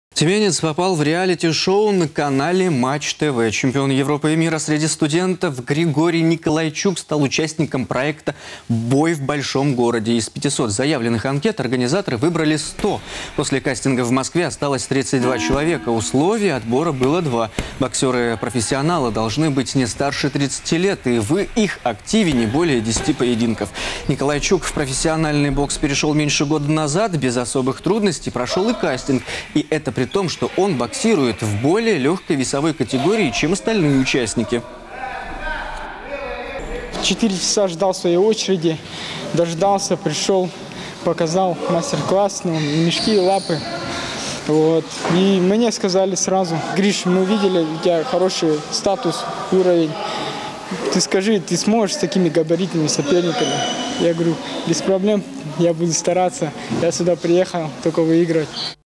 Тюменец попал в реалити-шоу на канале Матч-ТВ
Чемпион Европы и мчра среди студентов Григорий Николайчук стал участником проекта "Бой в большом городе". Из Пятисот заявленных анкет, организаторы выбрали 100. После кастинга в Москве осталось 32 человека. Условий отбора было два: боксёры-профессионалы должны быть не старше 30-ти лет и в их активе - не более 10-и поединков. Николайчук в профессиональный бокс перешёл меньше года назад. Без особых трудностей прошёл и кастинг. И это при том, что он боксирует в более лёгкой весовой категории, чем остальные участники